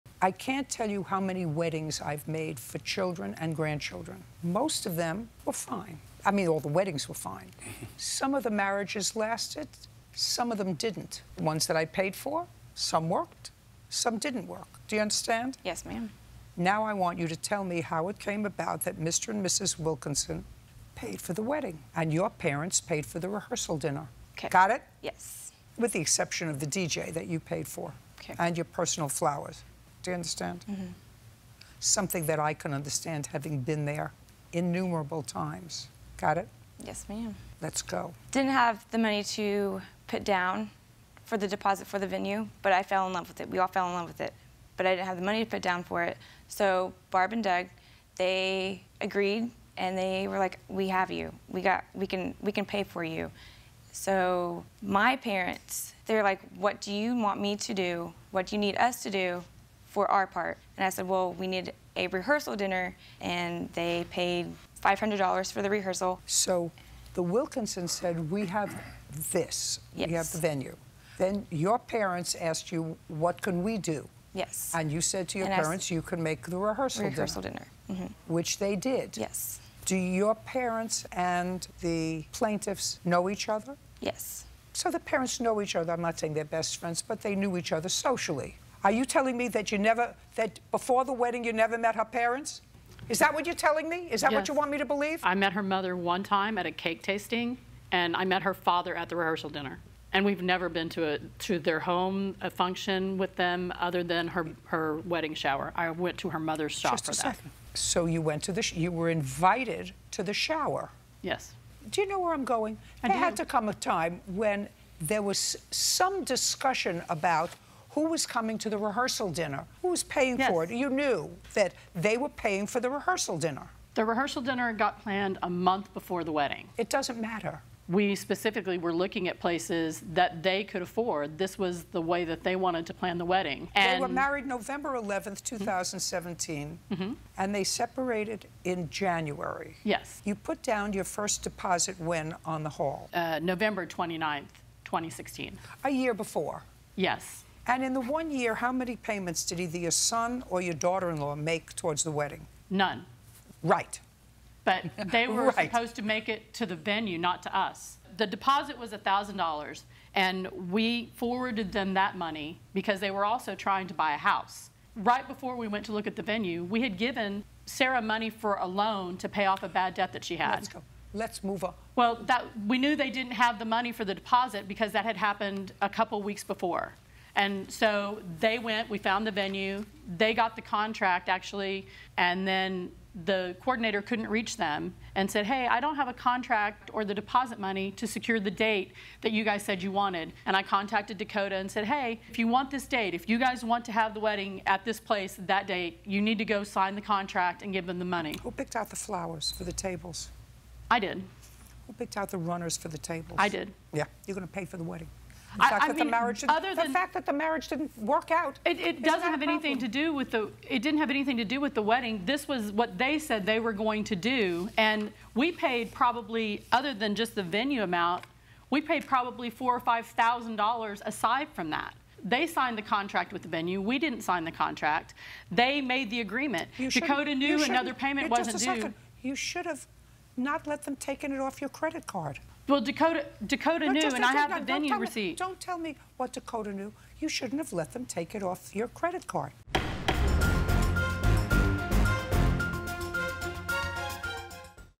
Parents Want Wedding Refund from Son! | Part 2
Dakota and Sara separated soon after their wedding, and Dakota's parents want their wedding money back!